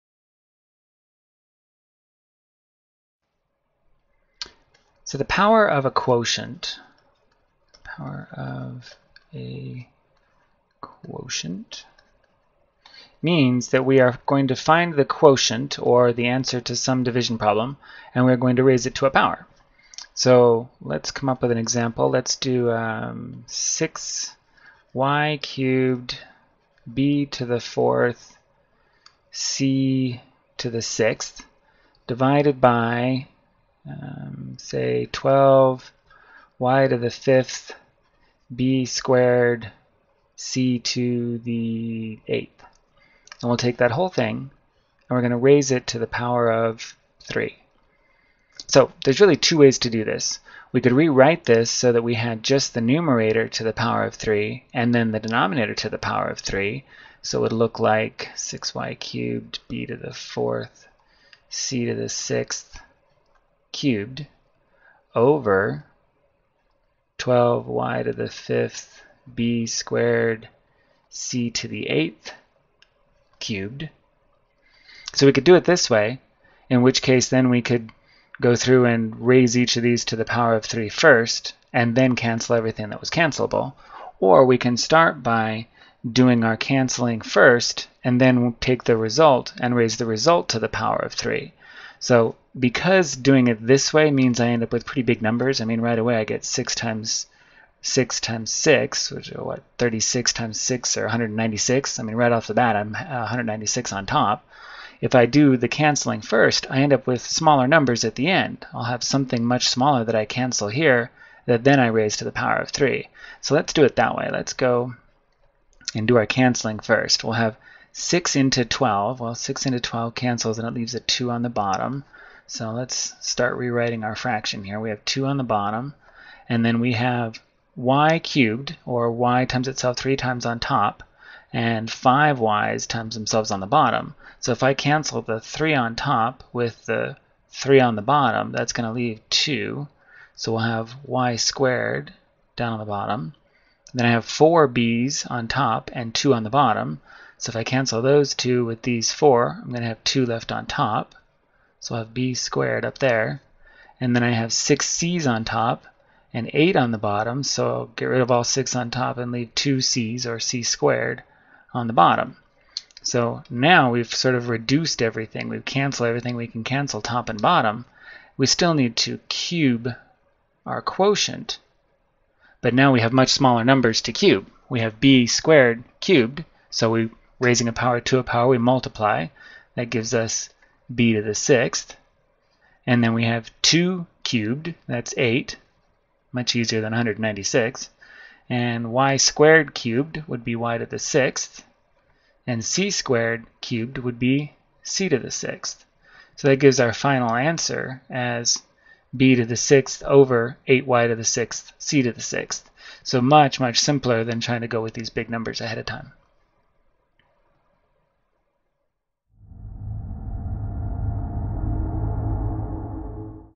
Power of a Quotient: An Application (Algebra I)
Here you'll learn how to simplify a fraction with exponential expressions in both its numerator and denominator that is raised to another secondary power.

This video demonstrates a sample use of the power of a quotient.

12. Applications of Exponential Functions